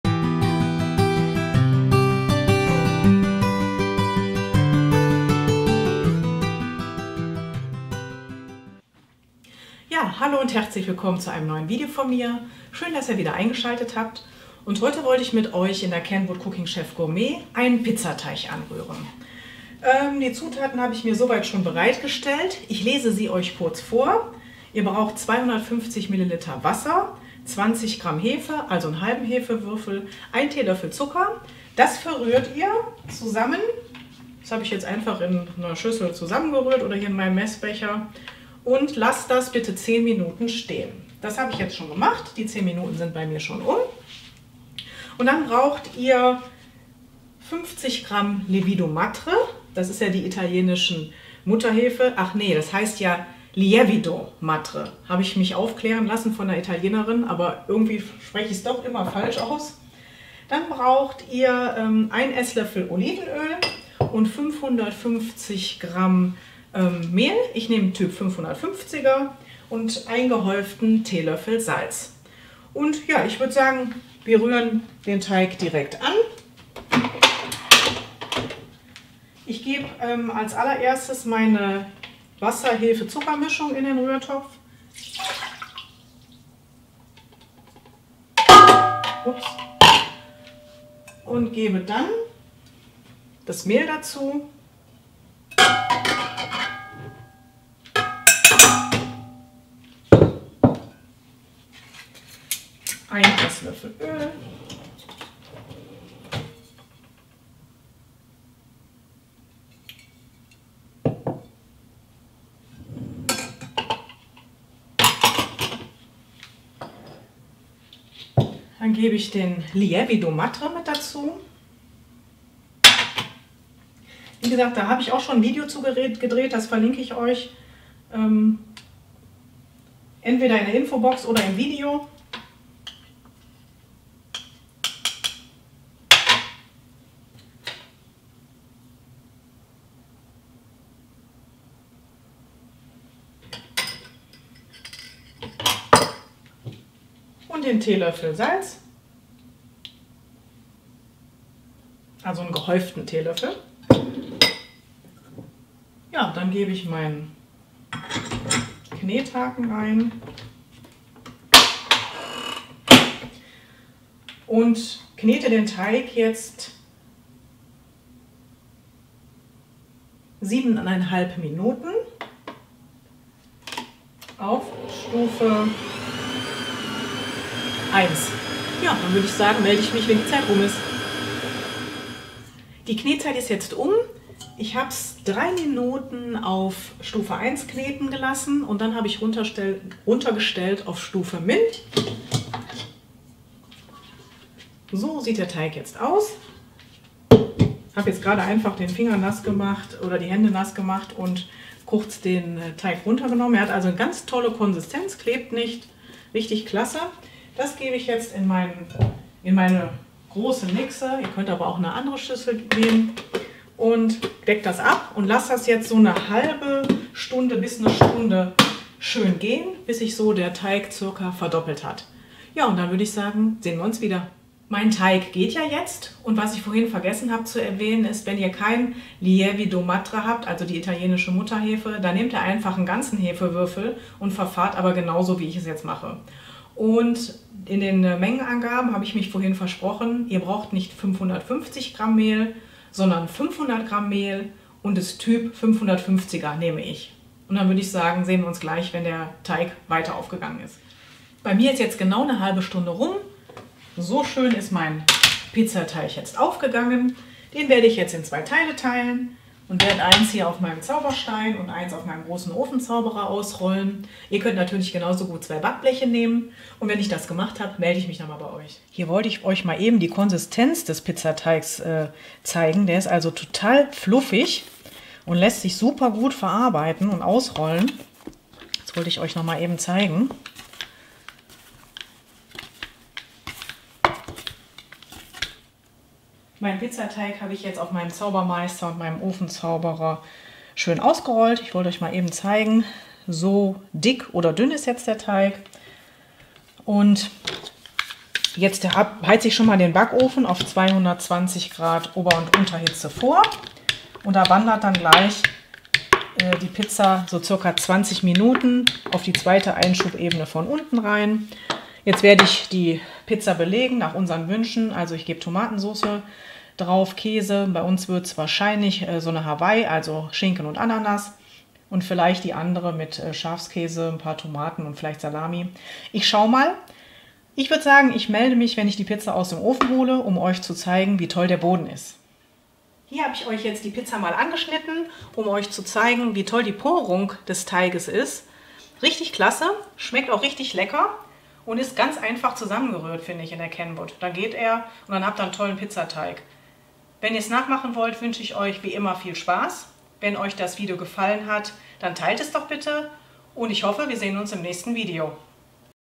Der perfekte Pizzateig - Kenwood Cooking Chef Gourmet
Hier zeige ich euch meinen liebsten Pizzateig, in der Kenwood Cooking Chef ist er ganz fix zusammen gerührt.

Hier sind die Zutaten für zwei Backbleche oder die Stoneware von Pampered Chef (großer Ofenzauberer oder Zauberstein)

250ml kaltes Wasser
20g frische Hefe (1/2 Hefewürfel) - 40g frische Hefe ohne Lievido madre
1 TL Zucker

-- in die Rührschüssel geben, miteinander verrühren und 10 Minuten stehen lassen

50g Lievido madre (ital. Mutterhefe)
1 EL Olivenöl
1 geh. TL Salz
500g Mehl Type 550er

-- zum Hefe-/Zuckerwassergemisch geben, Knethaken einsetzen und 3:30 Minuten / Stufe 1 kneten, auf Stufe min  stellen und weitere 4 Minuten kneten

-- Den Teig abgedeckt ca. 1/2 - 1 Stunde gehen lassen, bis er sich verdoppelt hat

-- Teig auf zwei Backblechen oder zwei Zaubersteinen/Ofenzauberern verteilen und nach Belieben belegen

-- Backofen auf 220°C O/U vorheizen und ca. 20 -25 Minuten fertig backen.

Ich wünsche Euch viel Spaß beim nachmachen :-)